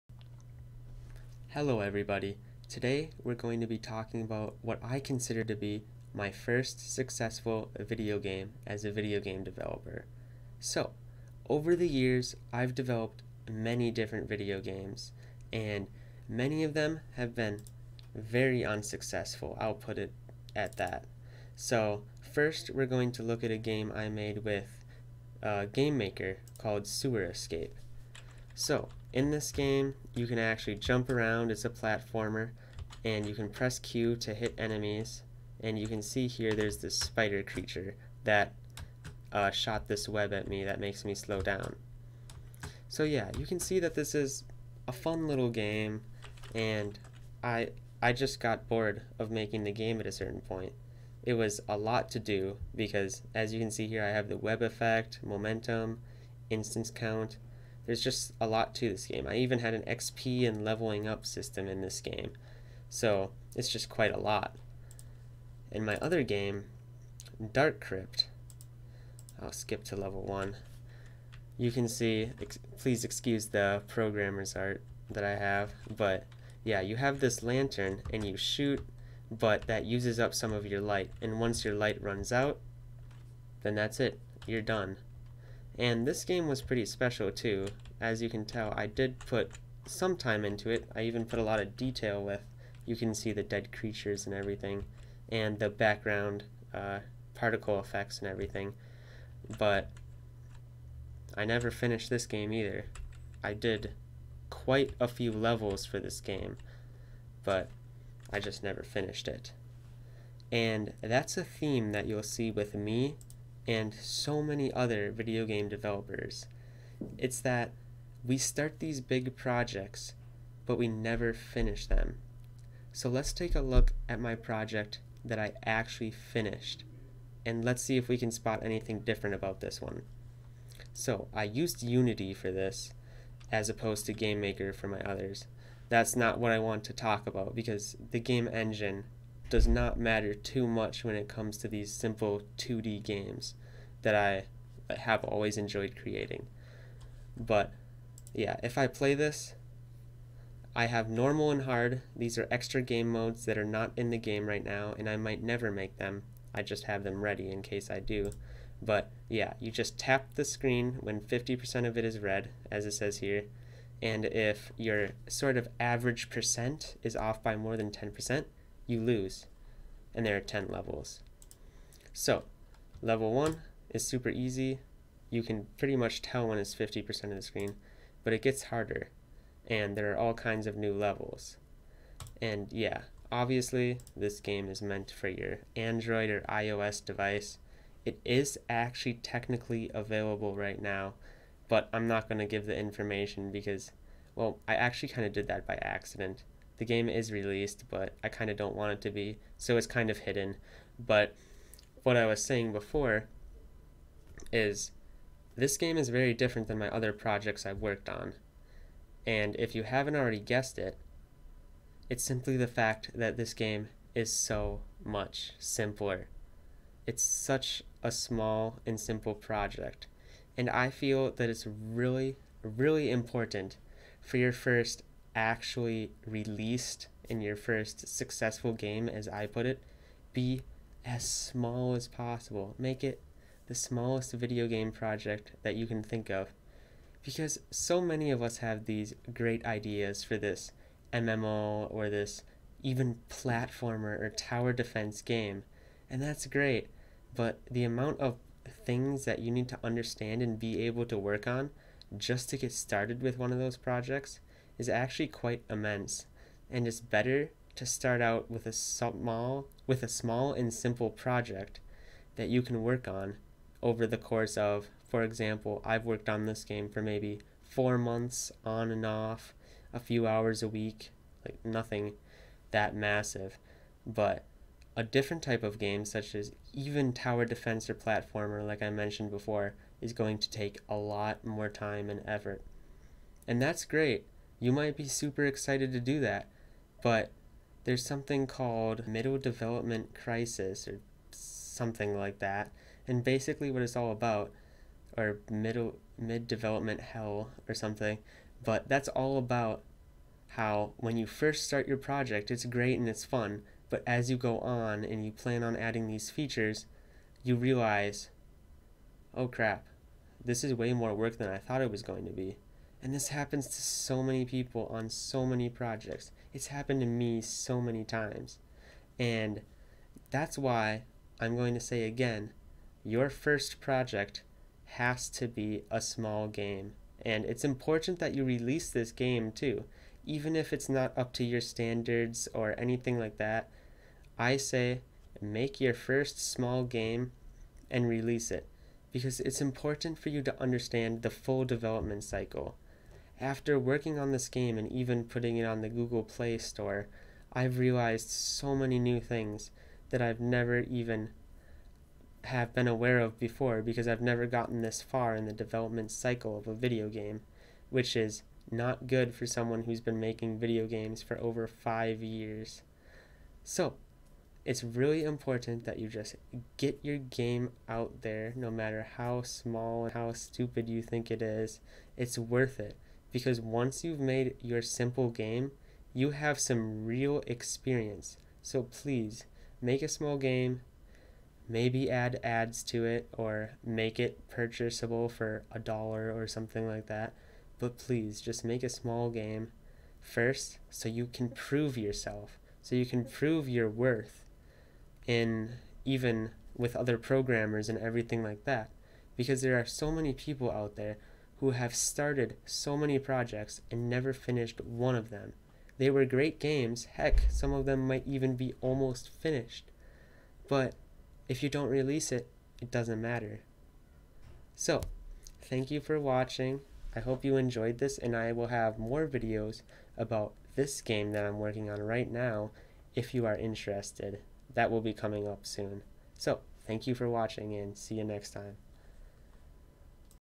How I Finished My (First Successful) Game Project
Making your first game is hard.  But there are many ways to make it easier, I look through many of my old game projects that can hopefully serve as great examples of how NOT to start a game project.  Video games are not easy to create, they take a lot of time and effort, but in the middle of development it is the hardest.

Thanks game dev underground for the initial video on this topic.  I made this video to detail my personal experience with this topic.

I must also note that the closest game project I ever had to successful was Resurgent Punch.  I don't consider it successful as it was so small in scope.